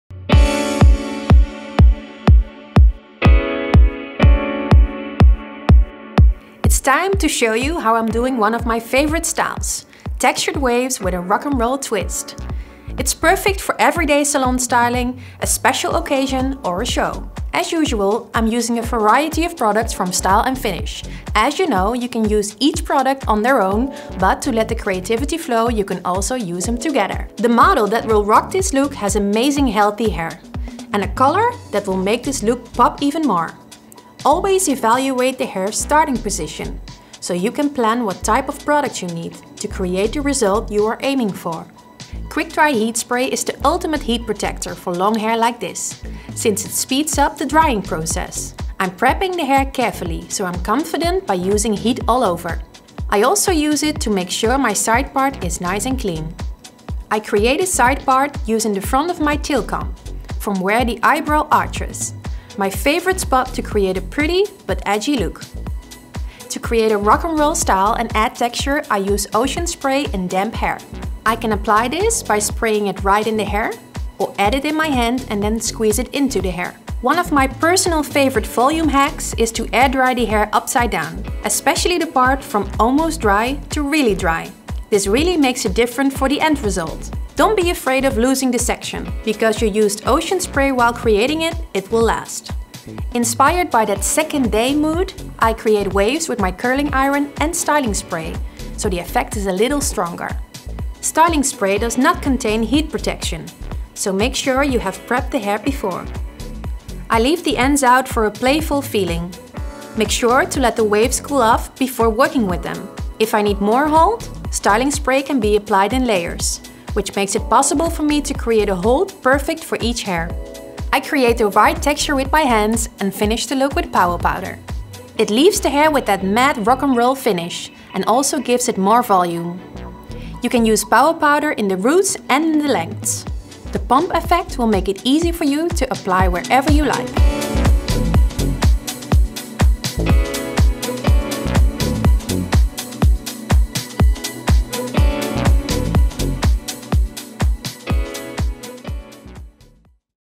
Styling Tutorial – Textured Waves
Master the textured waves! A look that works perfectly for everyday styling in the salon, special occasions or shows!